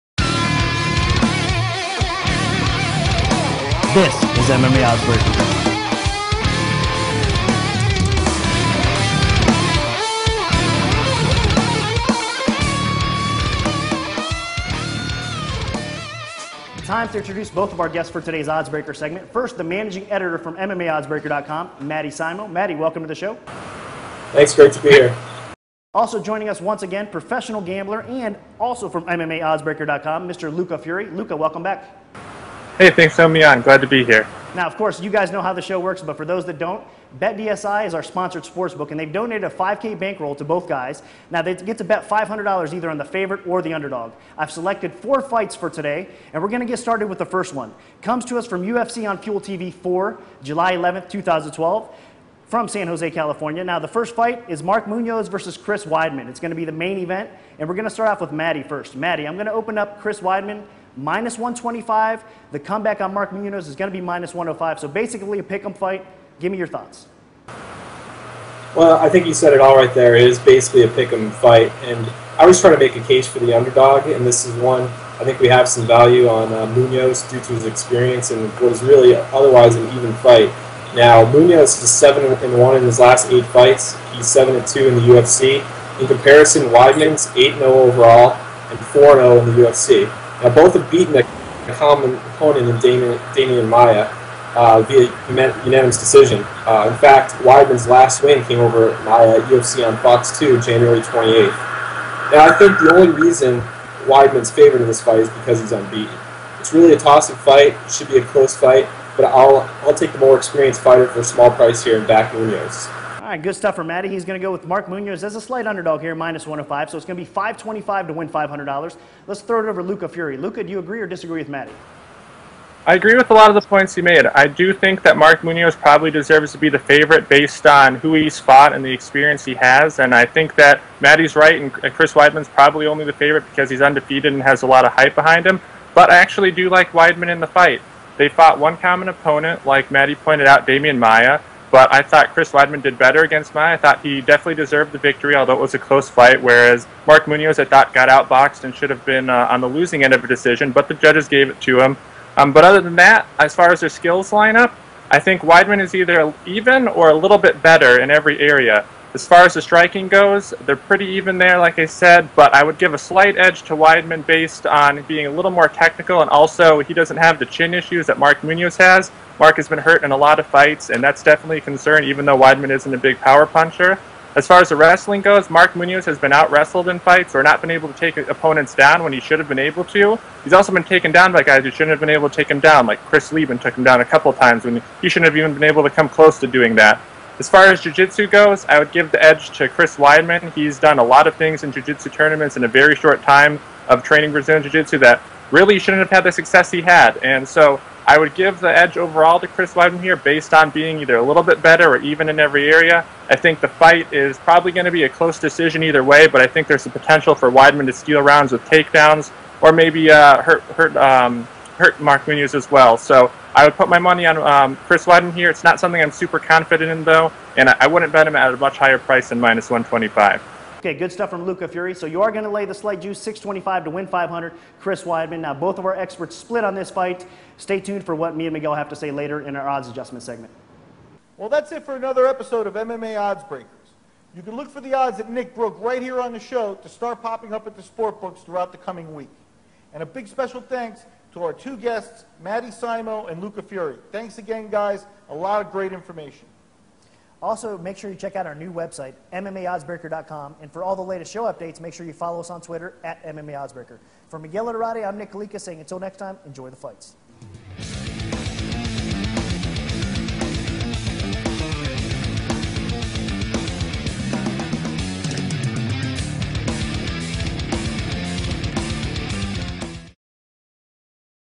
UFC on Fuel TV 4: Mark Munoz vs. Chris Weidman analysis and breakdown
UFC on Fuel TV 4 breakdown of th emain event between Mark Munoz and Chris Weidman

for UFC odds and to bet on In-Fight MMA Odds WHILE the fight is going on!